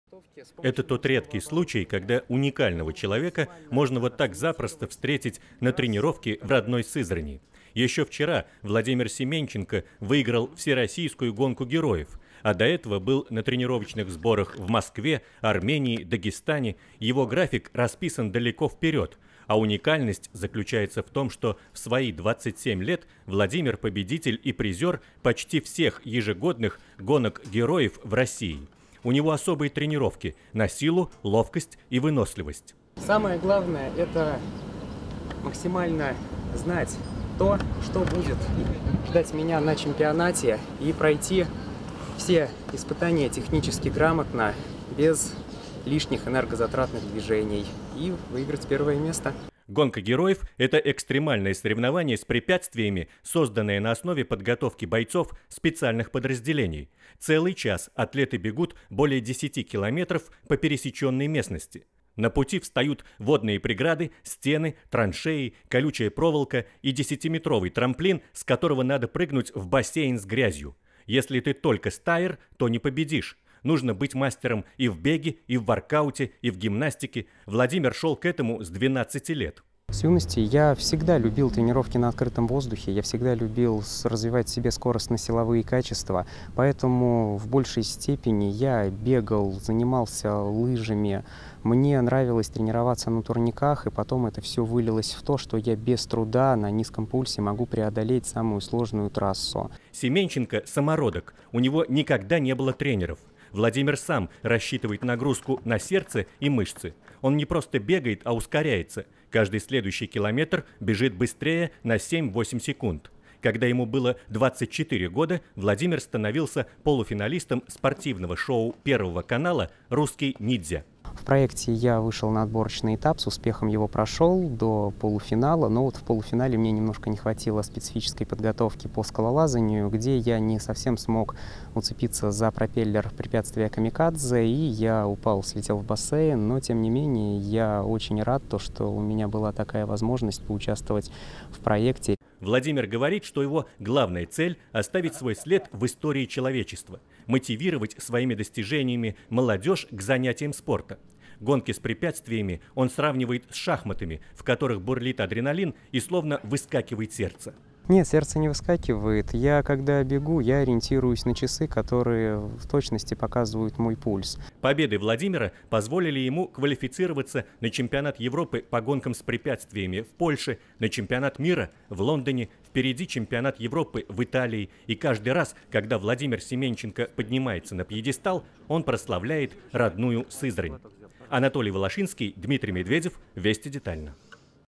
Русский ниндзя победил почти в каждой Гонке героев
Эксклюзивную тренировку показали съёмочной группе телевидения